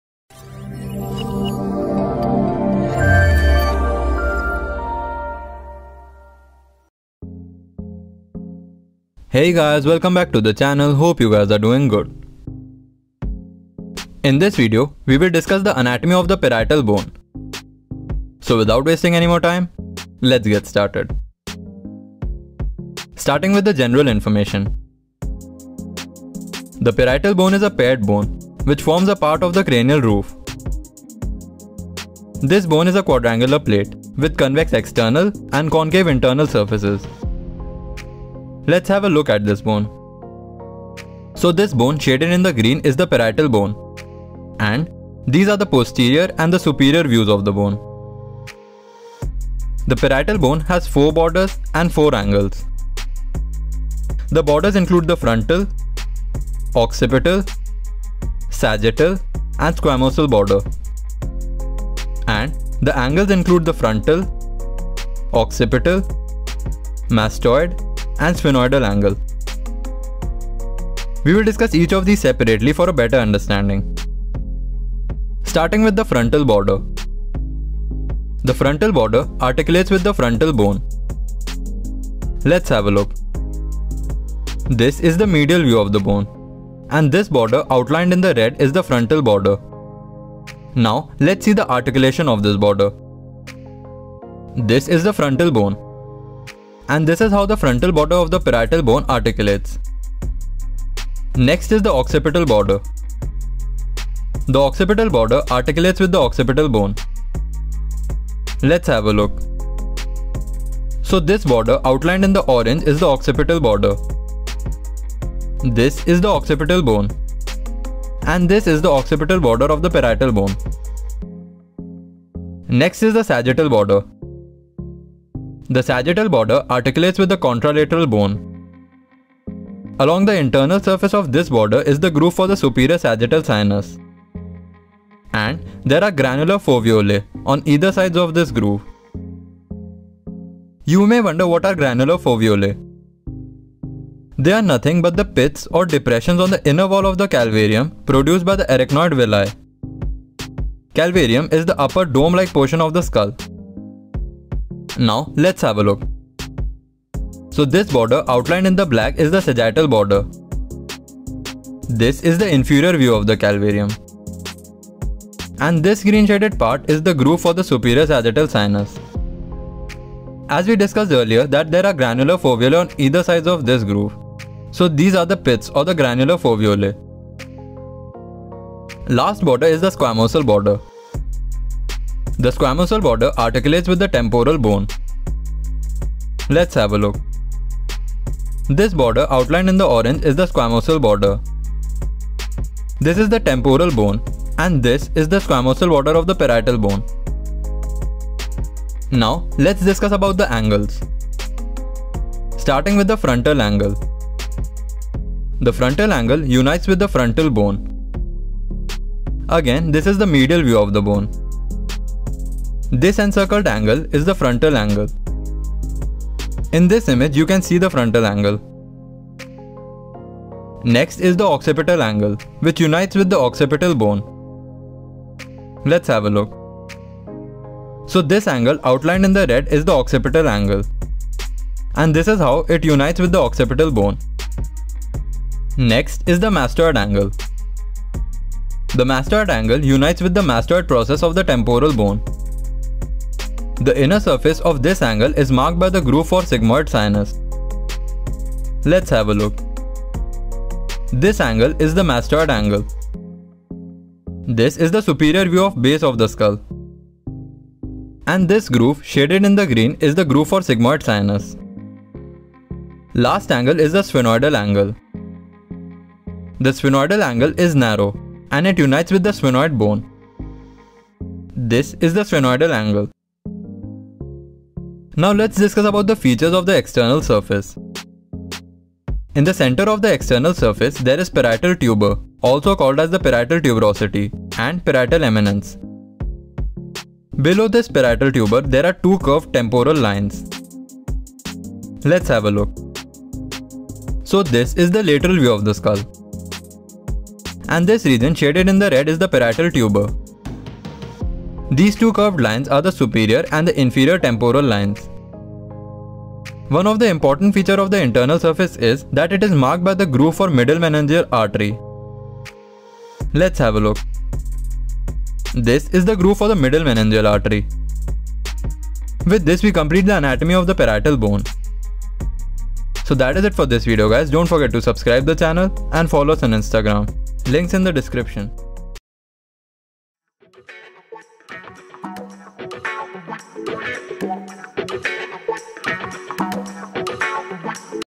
Parietal Bone Anatomy | Cranial Osteology #5
This video contains simplified explanation of the anatomy of parietal bone.

If you like our content then :-

By the Creator:-
Producing or re-uploading the video without the permission of the creator is subjected to copyright. ©️